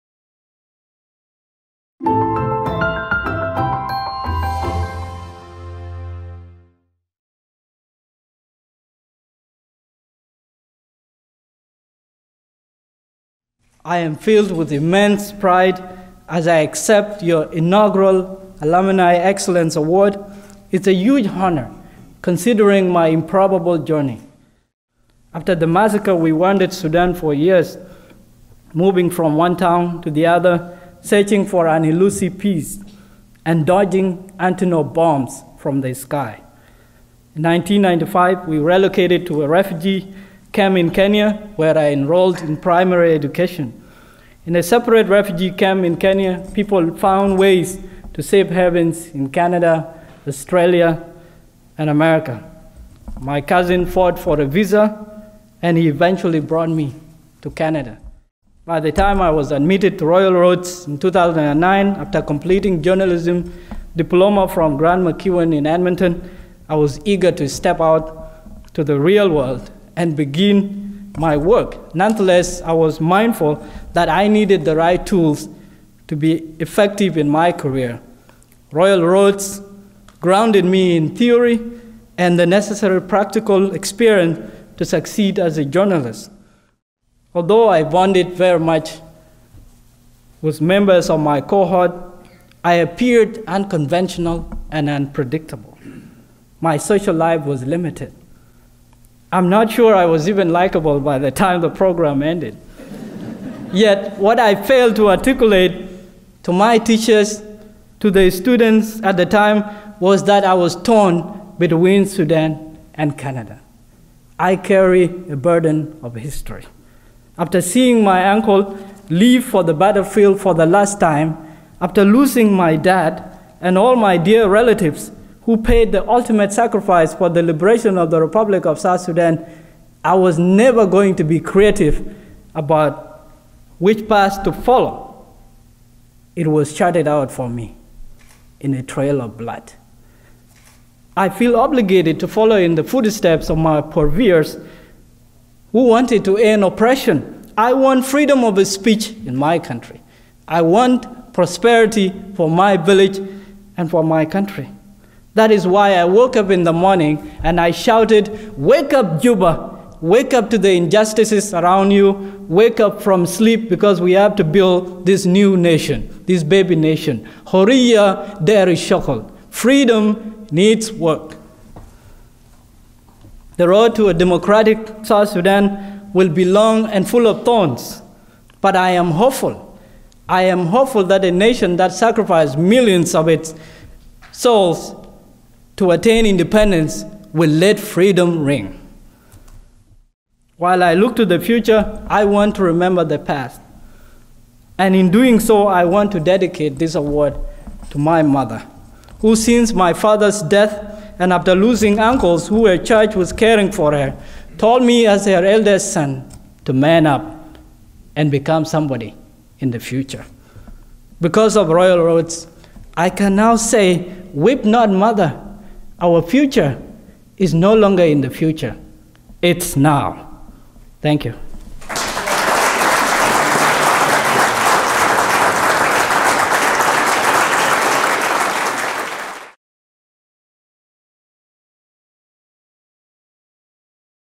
2012 Fall Convocation: Mading Ngor accepts his Alumni Excellence Award
Alumni Excellence Award winner Mading Ngor accepts his award Oct. 22, 2012.